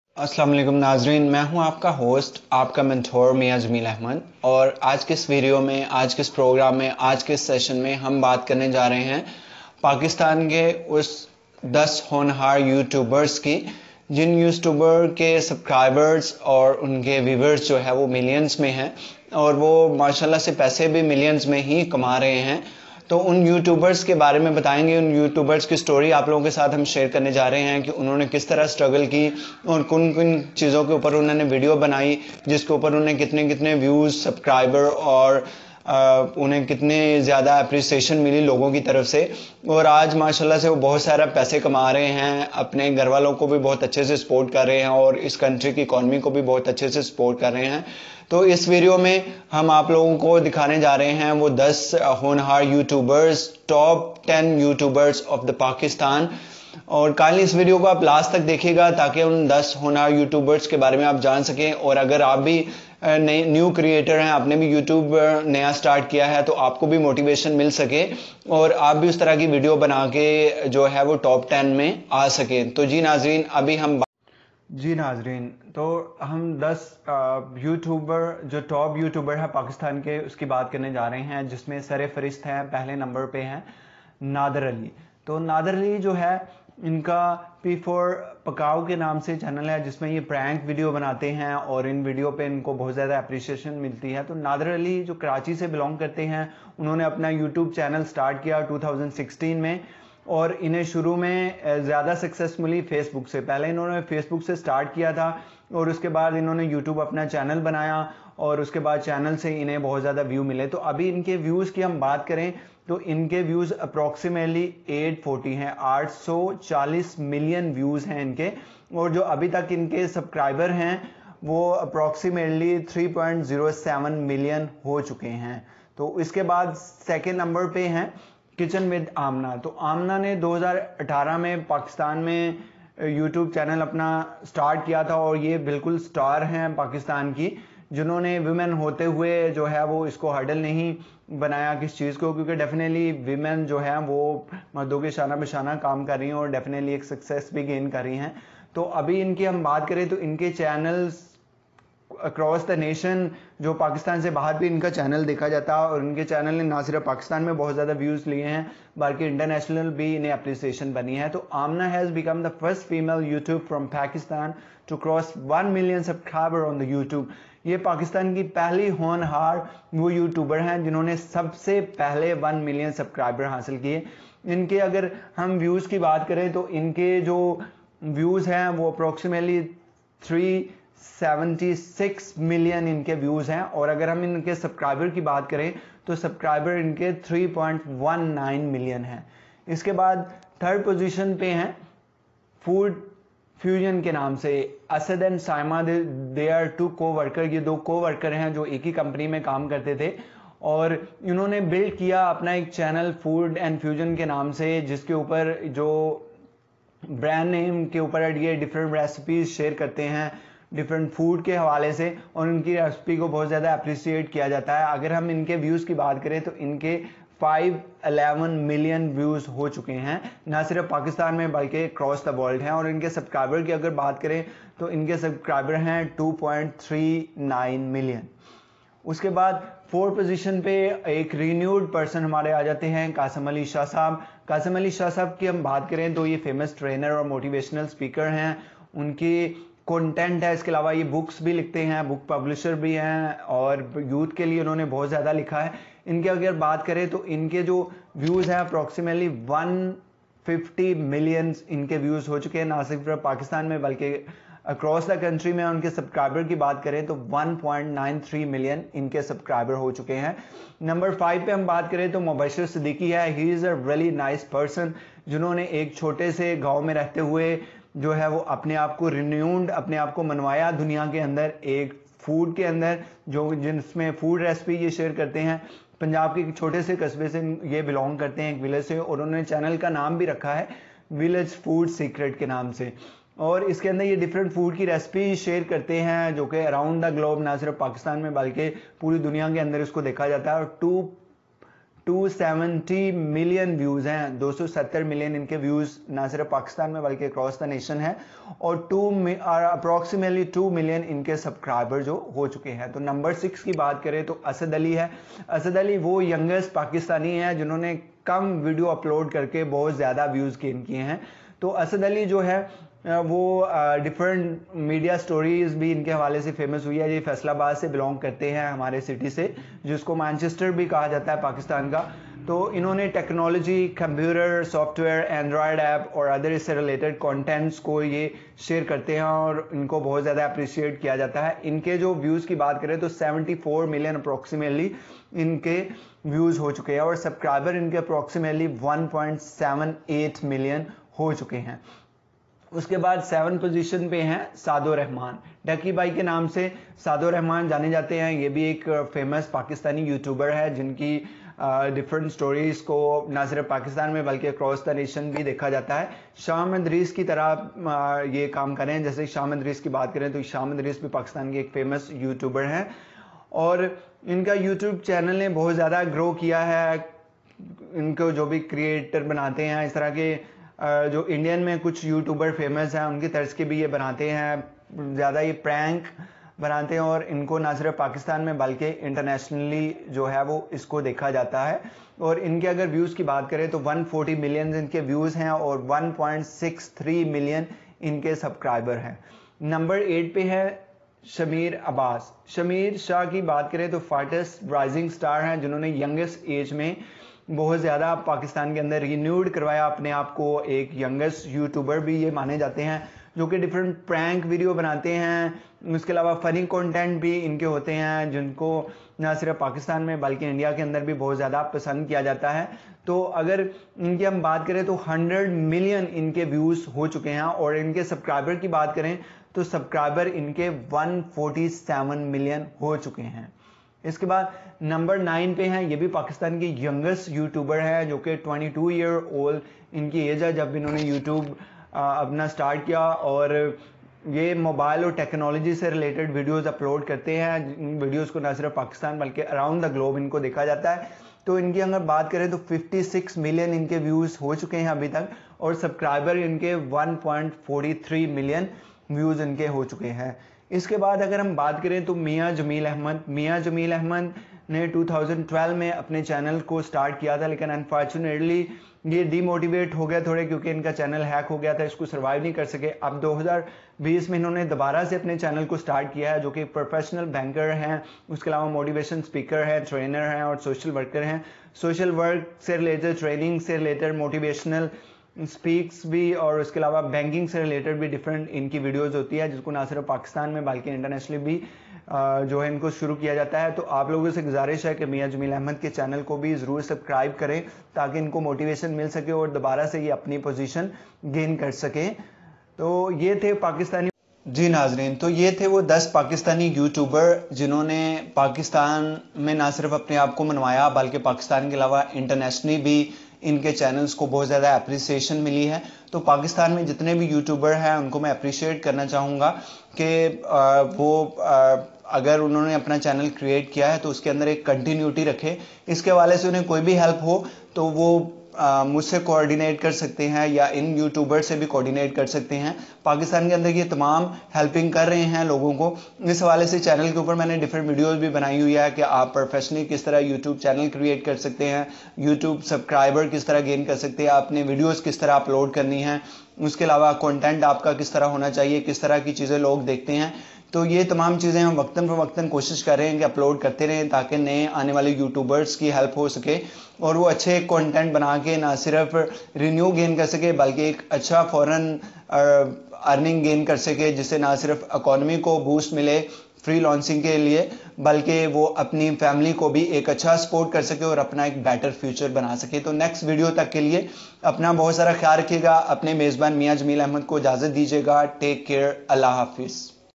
Top 10 youtubers in Pakistan / most viewed youtubers
Top 10 youtubers in Pakistan-most viewed youtubers in pakistan with their views and Subscribers